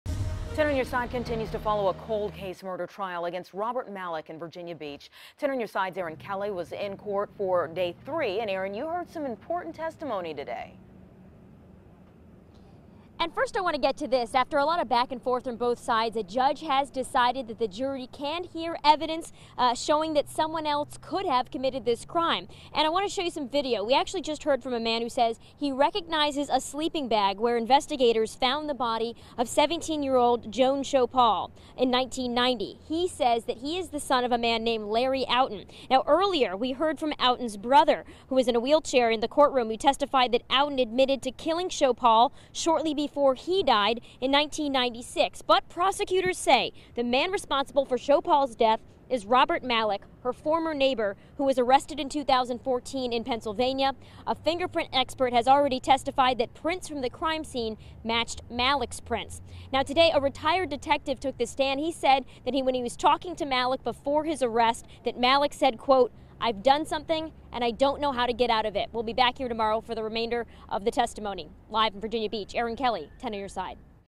Man says brother confessed in 1996 to murder in Malick case
10 On Your Side's Erin Kelly reports for WAVY News.